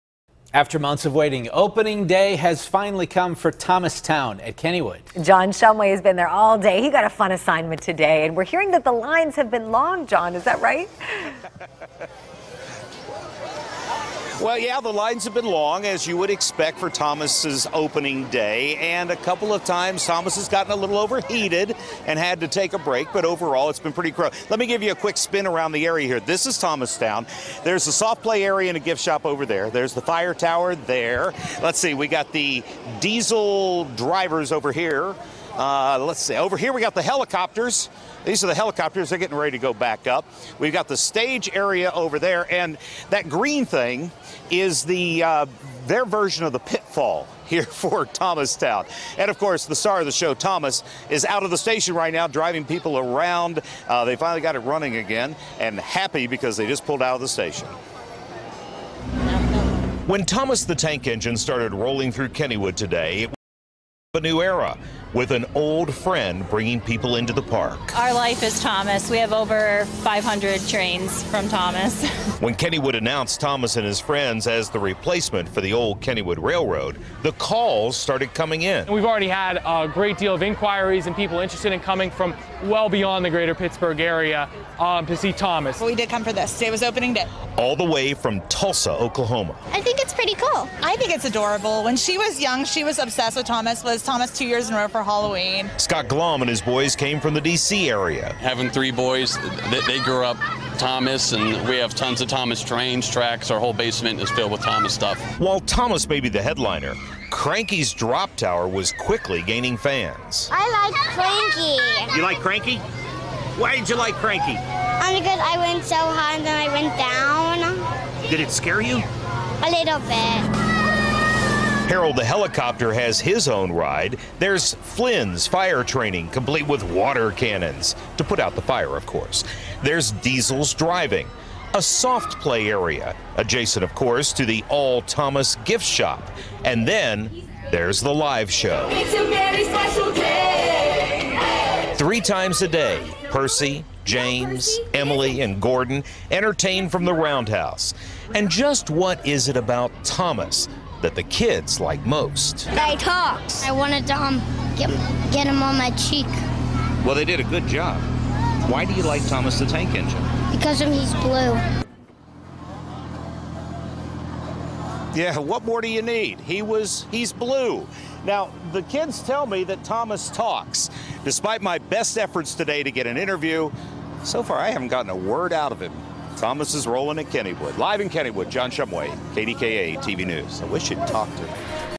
All Aboard! Thomas Town Officially Opens At Kennywood
Thomas Town officially opened at Kennywood on Friday; KDKA's John Shumway reports.